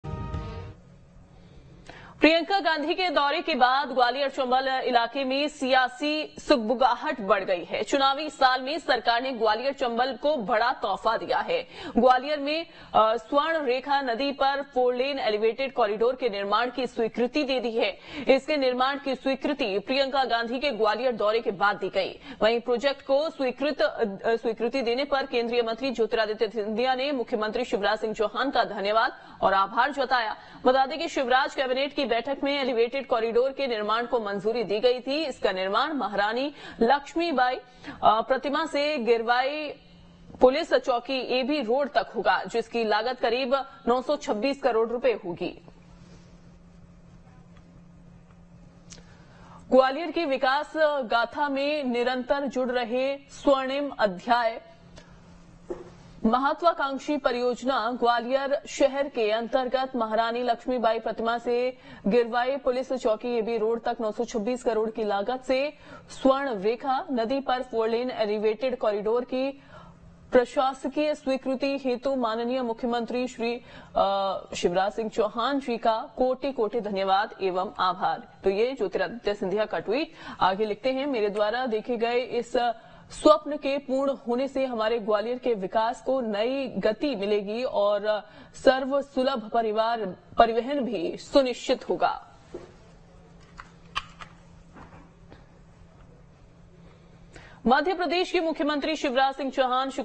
Gwalior में Fourlane Elevated Corridor को कैबिनेट की मंजूरी, Scindia ने जताया CM Shivraj का आभार
प्रियंका गांधी के दौरे के बाद ग्वालियर चंबल इलाके में सियासी सुगबुगाहट बढ़ गई है... चुनावी साल में सरकार ने ग्वालियर चंबल को बड़ा तोहफा दिया है... ग्वालियर में स्वर्णरेखा नदी पर फोरलेन एलिवेटेड कॉरिडोर के निर्माण की स्वीकृति दे दी गई है...इसके निर्माण की स्वीकृति प्रियंका गांधी के ग्वालियर दौरे के बाद दी गई...वहीं प्रॉजेक्ट को स्वीकृति देने पर केंद्रीय मंत्री ज्योतिरादित्य सिंधिया ने मुख्यमंत्री शिवराज सिंह चौहान का धन्यवाद और आभार जताया है...

After Priyanka Gandhi's visit, political buzz has increased in Gwalior Chambal area... In the election year, the government has given a big gift to Gwalior Chambal... Approval has been given for the construction of a four-lane elevated corridor on the Swarnarekha river in Gwalior...